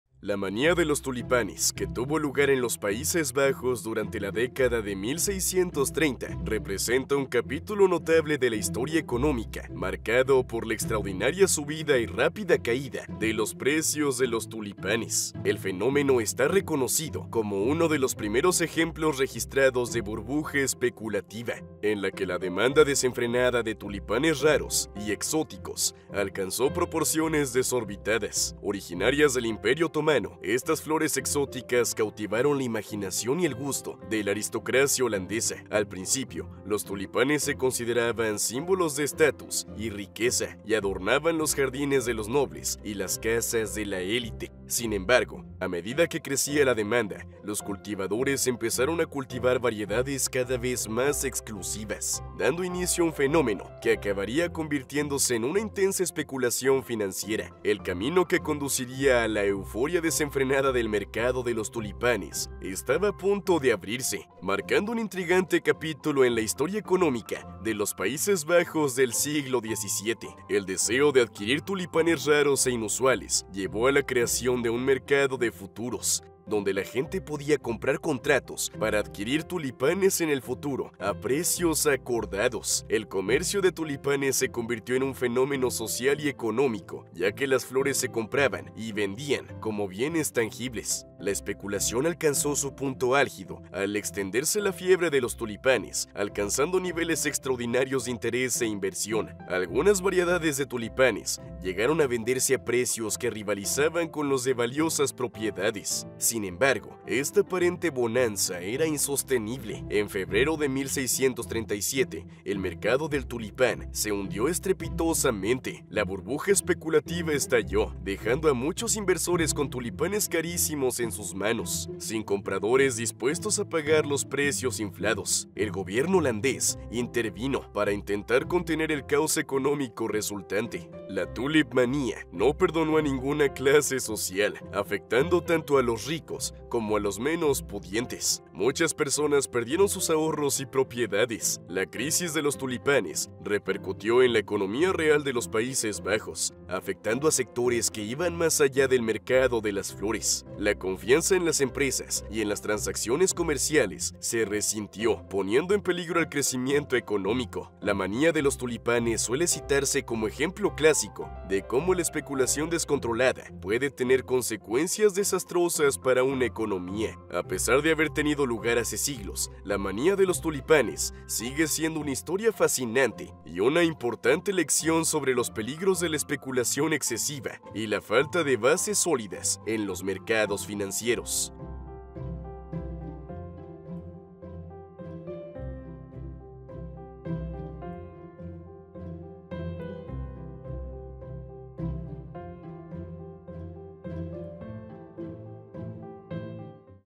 La Tulipomanía – La Gran Burbuja Económica Holandesa
Los tulipanes en los Países Bajos son un símbolo nacional y una gran atracción turística, especialmente durante la primavera. Este fenómeno refleja la rica historia de los tulipanes en la economía y cultura holandesa, que se remonta al siglo XVII cuando la " Crisis de los Tulipanes " causó que los precios de los tulipanes se dispararan.

Mira la Historia